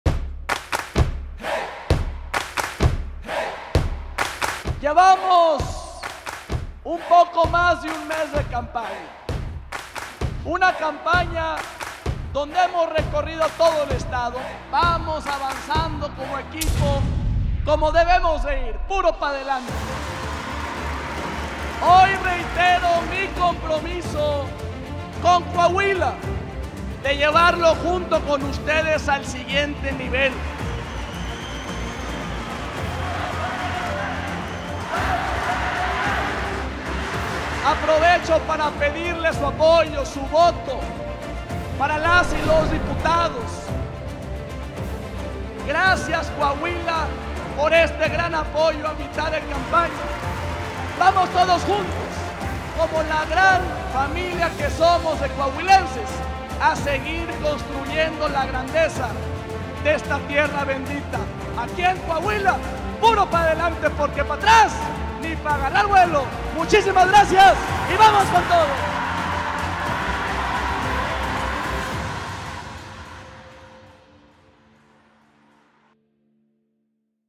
Manolo Jiménez resumen un mes de campaña
¡Vamos con todo! ¡Qué gusto saludar a nuestra gente de La Laguna! Con un extraordinario evento en Torreón seguimos redoblando esfuerzos, acelerando y avanzando. PaDelanteCoahuila VamosAGanar👍🏼🤠💪🏼